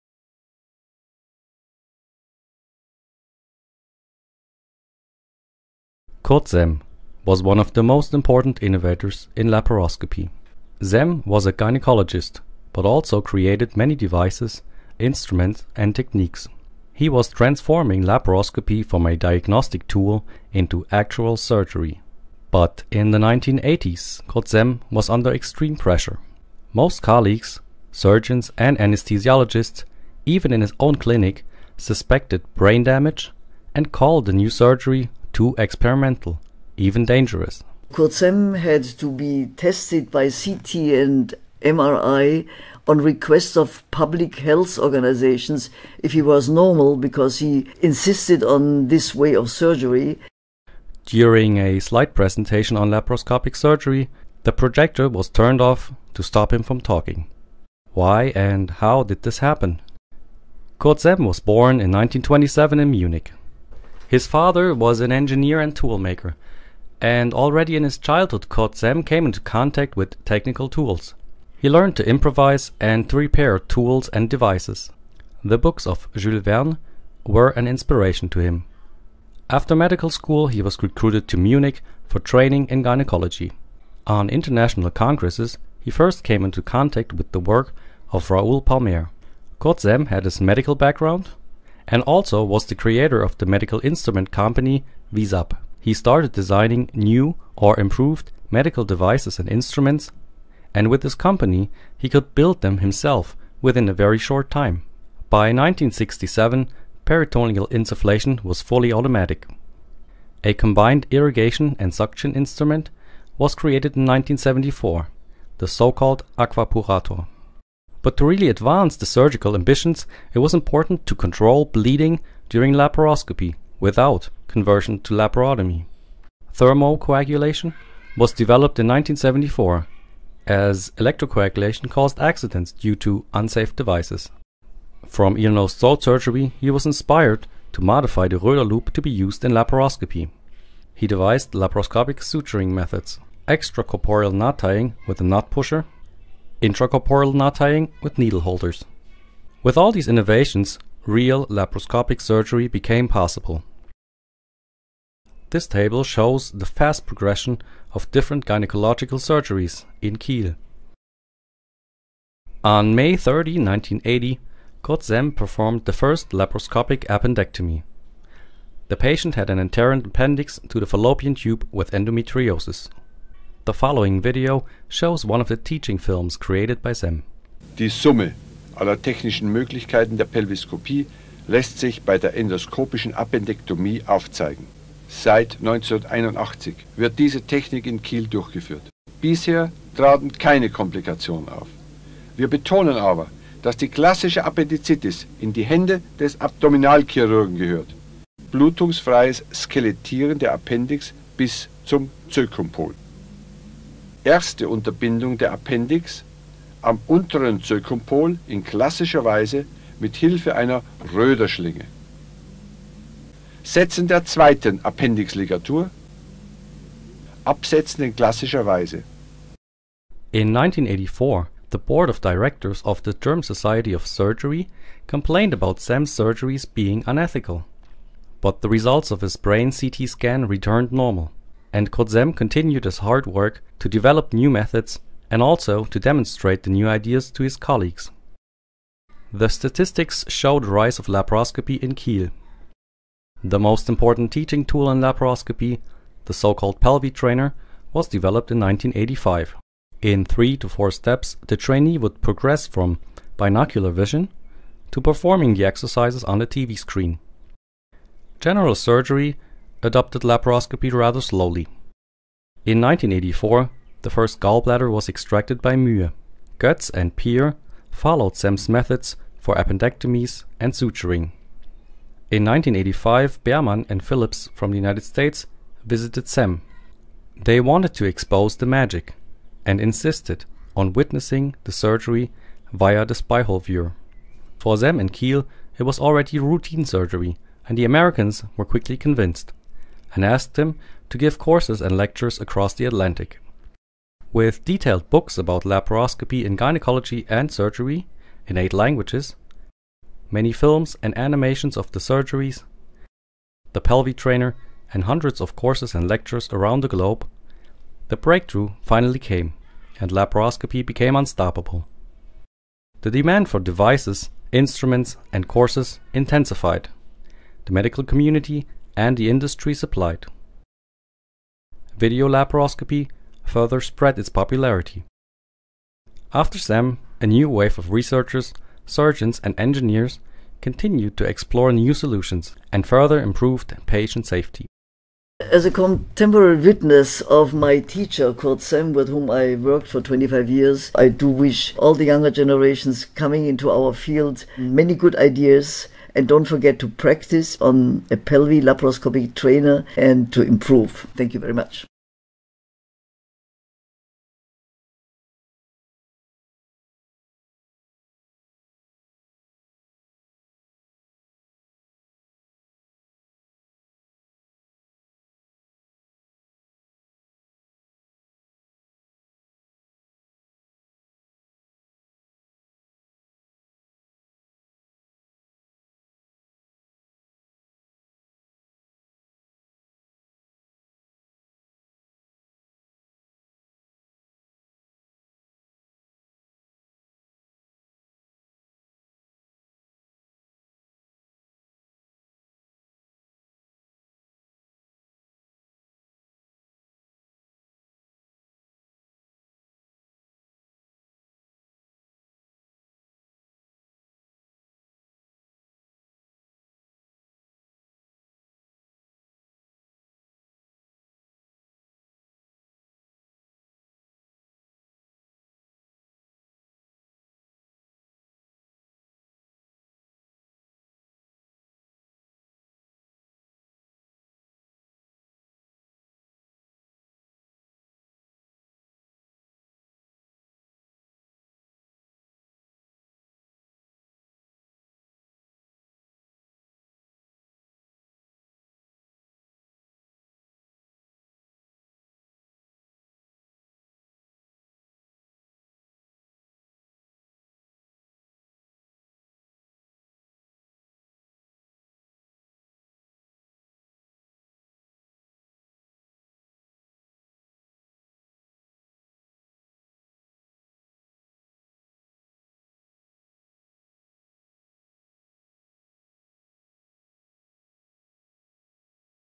Kurt Semm - Innovator and Teacher of Laparoscopic Surgery
A research film project in surgical history. 2016.